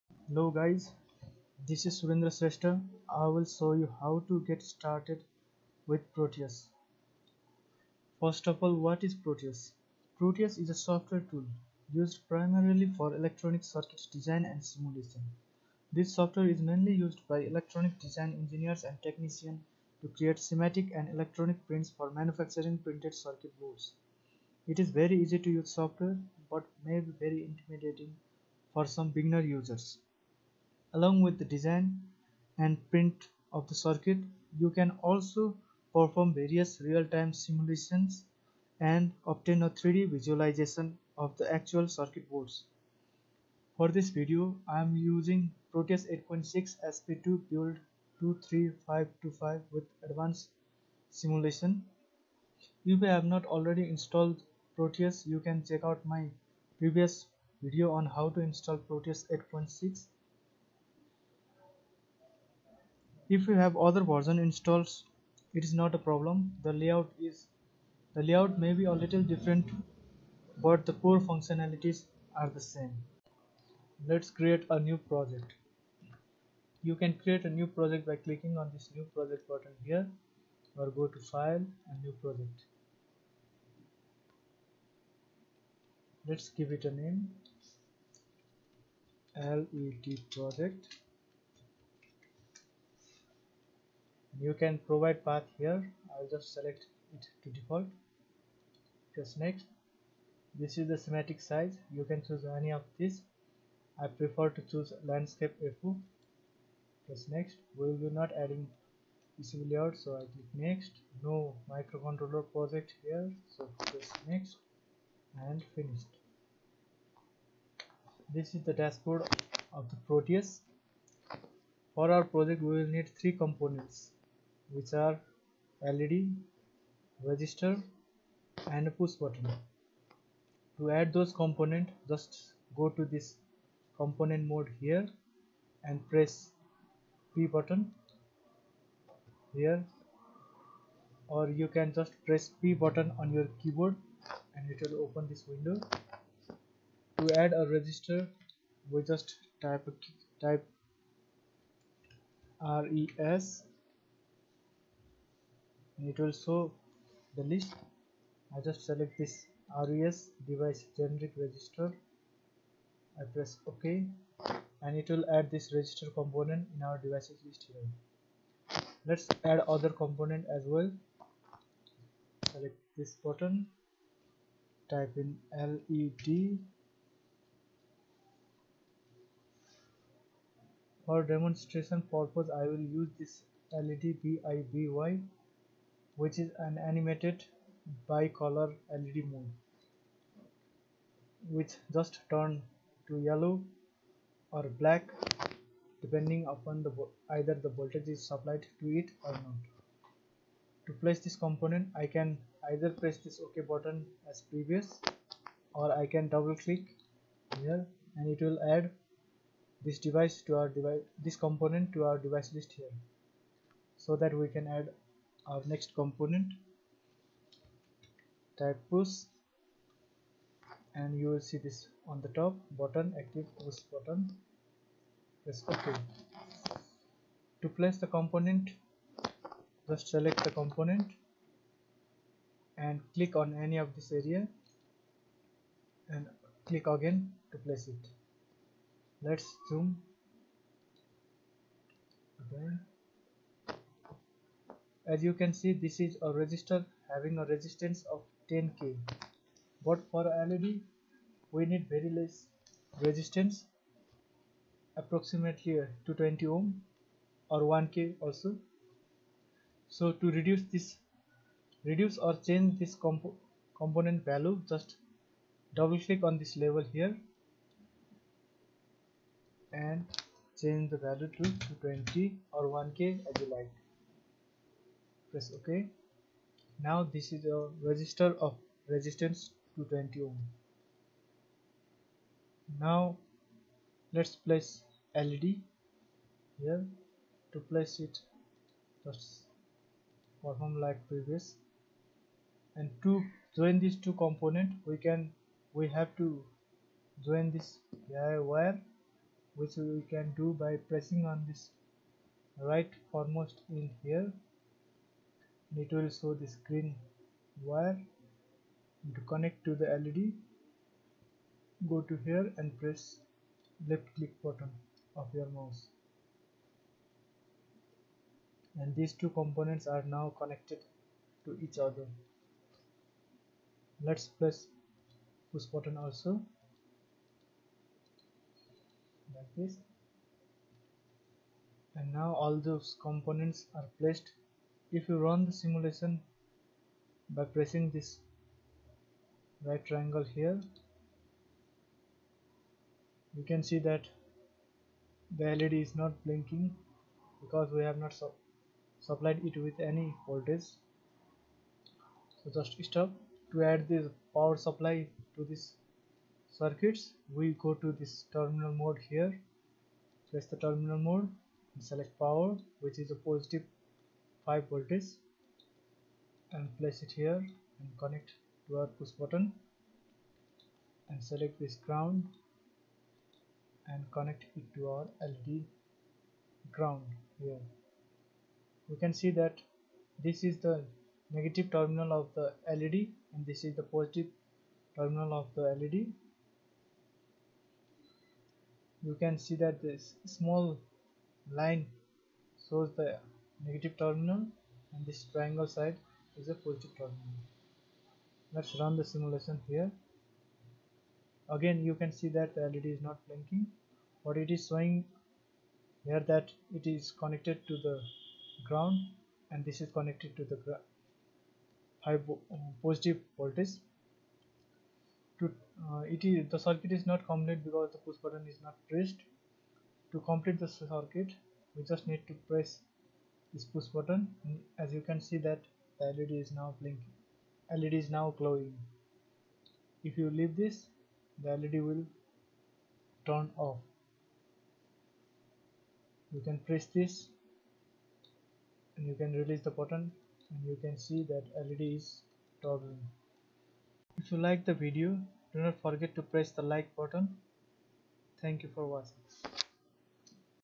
Proteus Tutorial - Basics - Led Project
A brief tutorial on creating a simple Led project on proteus.